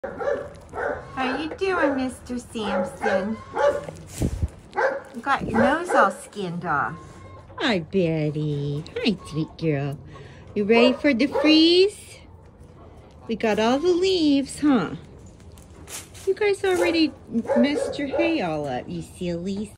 Preparing for the arctic blast
All fluffed in the cold❤️/Nigerian dwarf goats